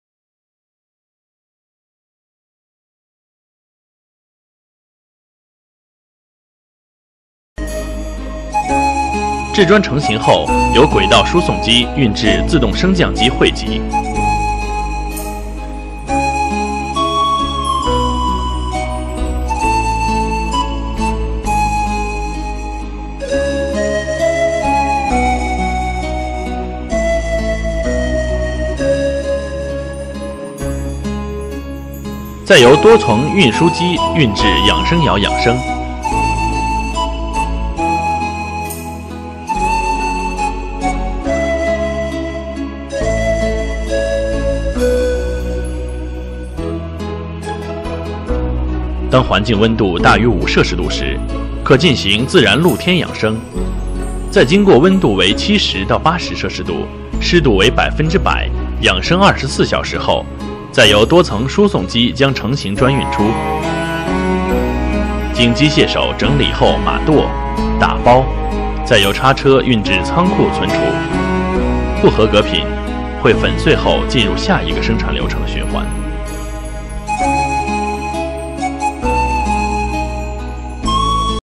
Brictec Automatic Brick Production line
This machine is applied to place fired brick stack on pallet from kiln car, then conveyer will transport bricks to forklift. It is controlled by PLC system to make sure the whole process stable and reliable. The traveling and lifting operations of gripping hand is stable, and positioning is accurate. By adjusting the size of gripping hand, it available for many applications.

if you are interested, please consult us freely, my Skype is : brictec06